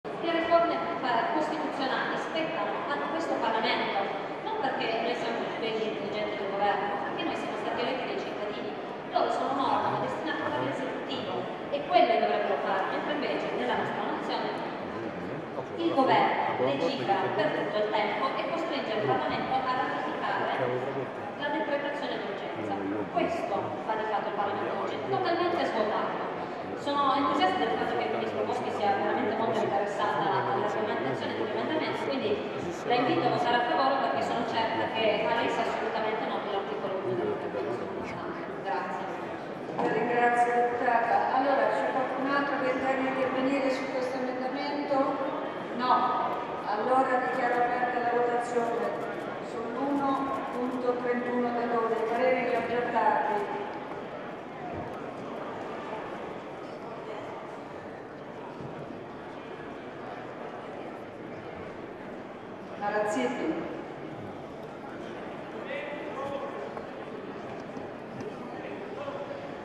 MARIA ELENA BOSCHI IN AULA CAMERA PARLOTTA CON GUERINI IMMAGINI 13 Gennaio 2015
(Agenzia VISTA) - ROMA, 13 Gennaio 2015 - In aula alla Camera il ddl riforme costituzionali.